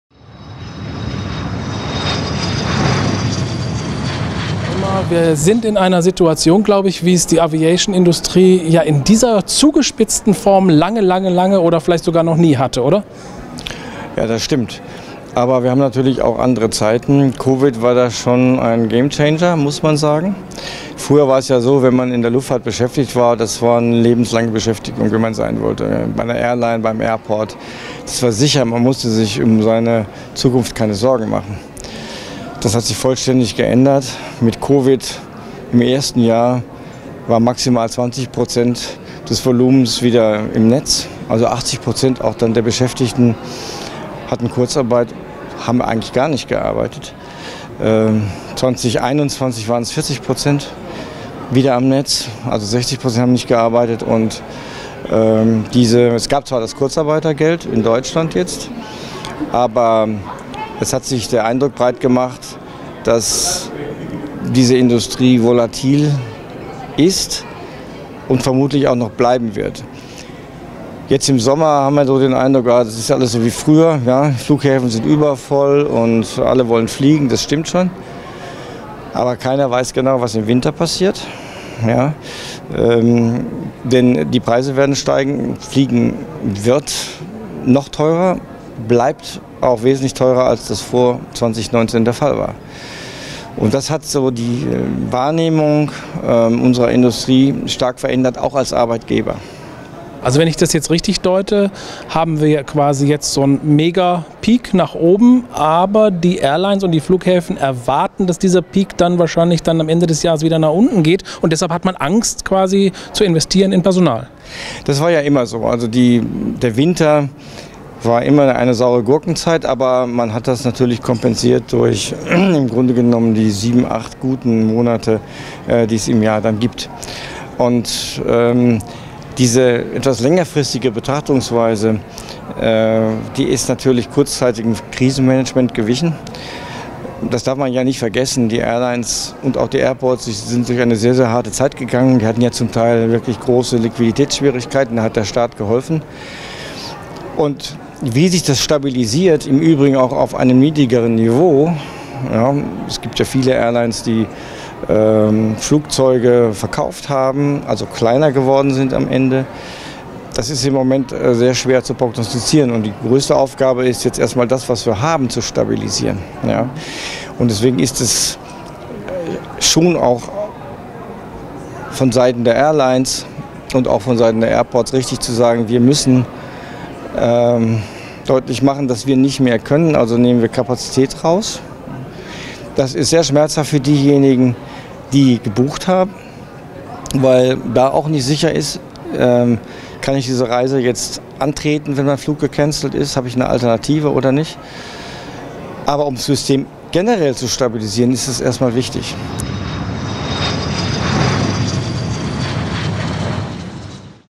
Luftfahrtexperte: Es gibt weiterhin große Sorgen um fehlendes Personal in der Luftfahrtbranche!
Wenn es einer wissen muss, dann Dr. Michael Kerkloh (ehemals Flughafenchef in München, derzeit im Lufthansa Aufsichtstrat und mit weiteren wichtigen Ämtern der Lufthartbranche verbunden…Dr. Kerkloh sieht es als richtig an, nun Kapazität aus dem Flugangebot herauszunehmen und spricht mit Sorge über die Personalknappheit in der Branche…